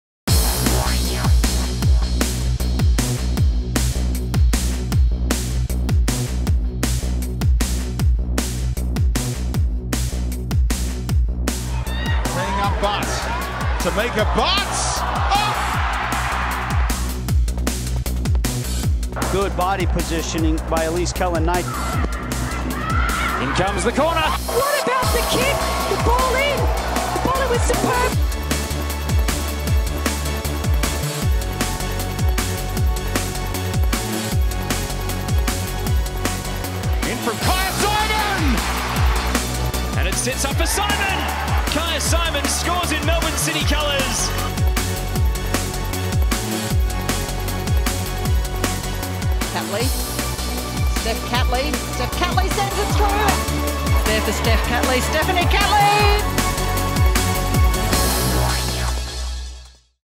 W-LEAGUE | City's Incoming Matildas
From Green and Gold to City Blue! We're delighted to announce the signings of Matildas stars Elise Kellond-Knight and Tameka Butt, as well as the return of Steph Catley and Kyah Simon ahead of the 2018/19 W-League season.